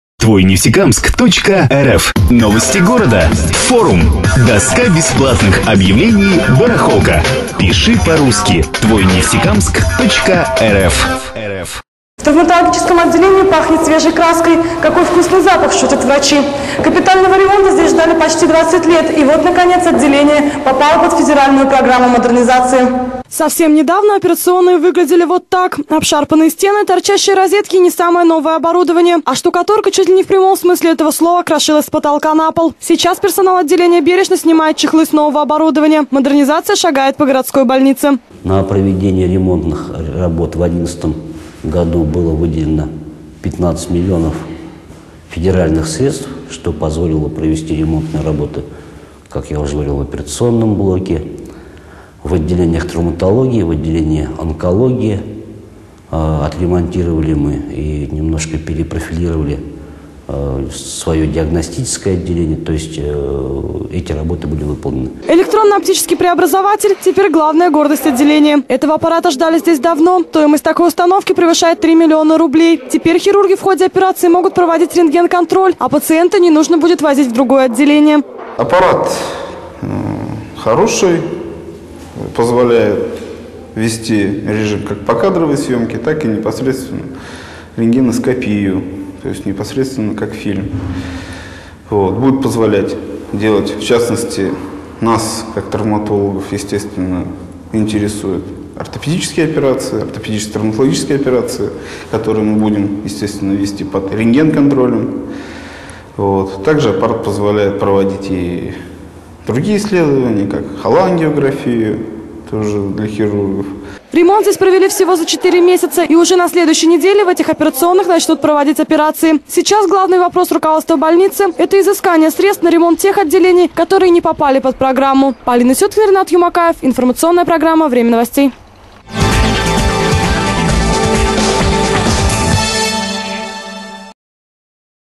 Модернизация шагает по городской больнице
Процесс непонятного и далекого слова модернизация в Нефтекамске можно посмотреть и даже пощупать. На этой неделе наводят лоск и запускают работу 4 отремонтированных операционных травматалогического отделения. На что были потрачены федеральные деньги - выясняла Полина Сюткина.